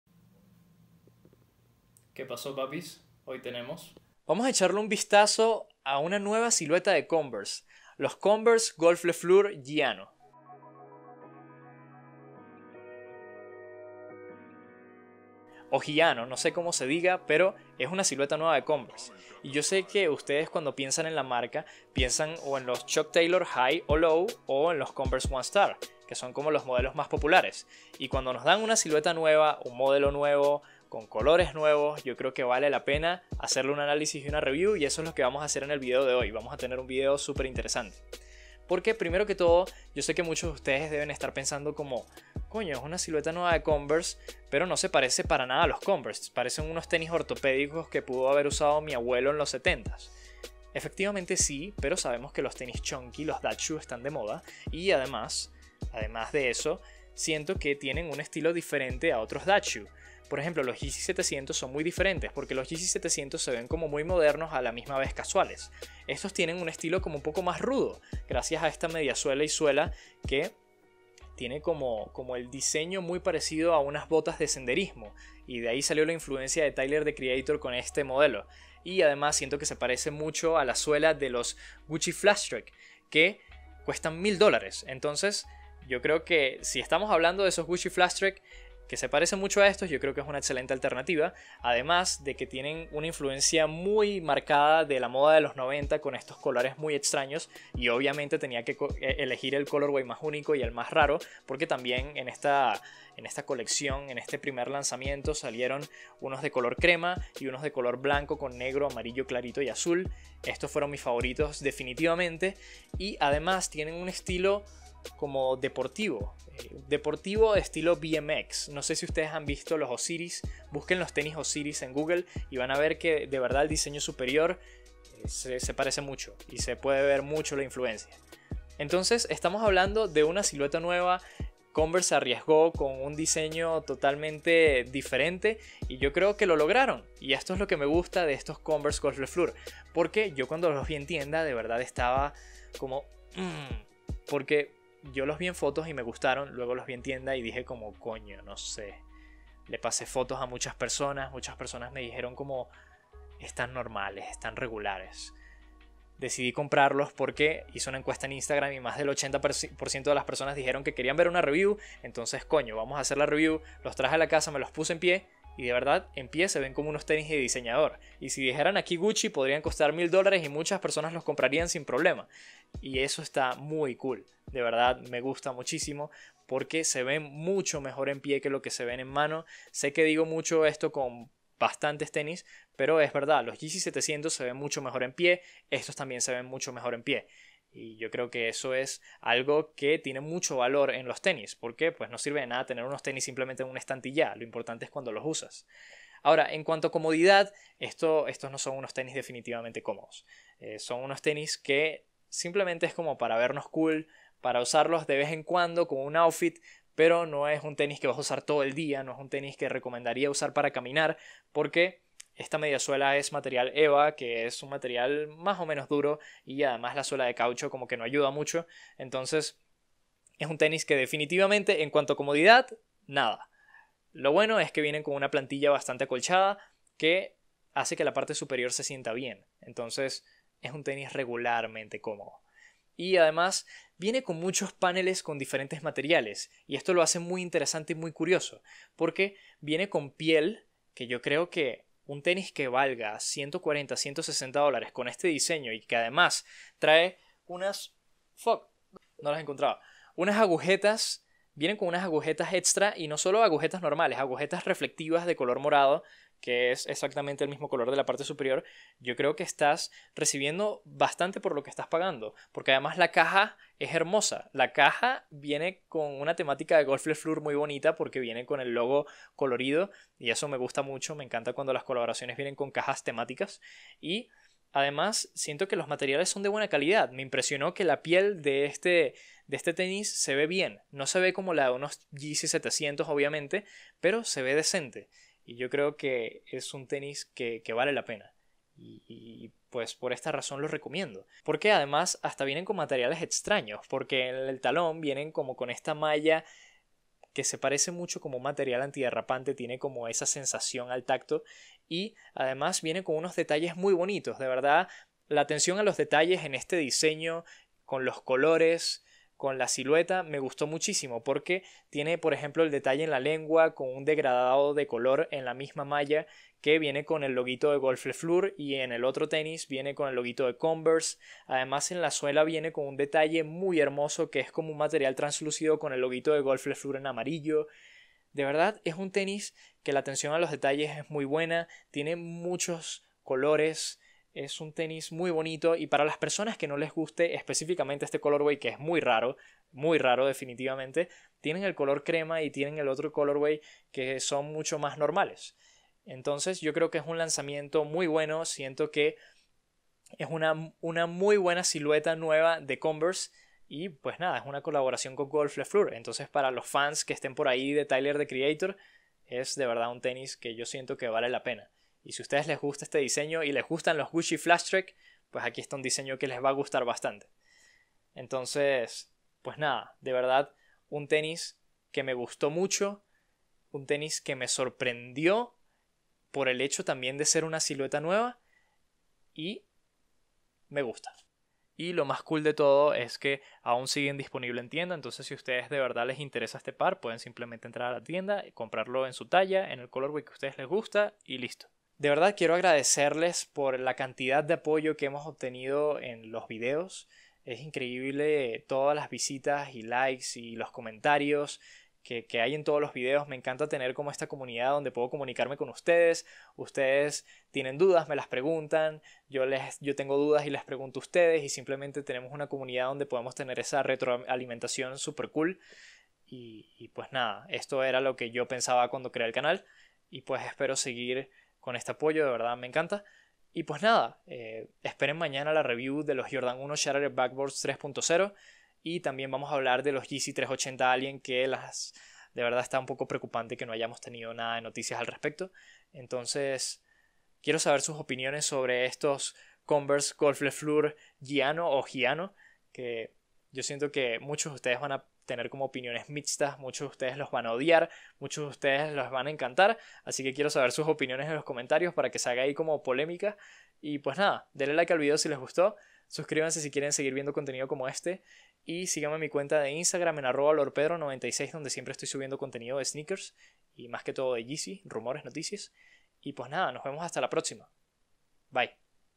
¡UNA NUEVA SILUETA! - Converse Golf Le Fleur Gianno Review/Análisis + en pie
Muy raro que Converse nos traiga una nueva silueta, en esta ocasión con Tyler the Creator con su marca Golf Le Fleur.

Inspirados en el DMX, La moda de los 90's y unas botas de Hiking nos llegan los Golf Le Fleur Gianno. Un tenis Chunky, estilo dad shoe con detalles muy interesantes

A ustedes que les parecen?

Mi Instagram: @lordpedro96